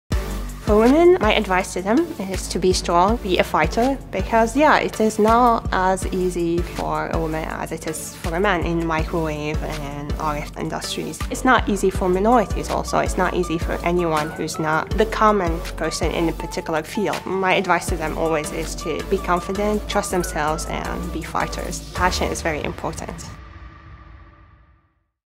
IMS2019 | Dina Katabi - Women in Microwaves
Dina Katabi, closing keynote speaker at IMS2019, talks about her experience as a woman in the microwave and RF industry.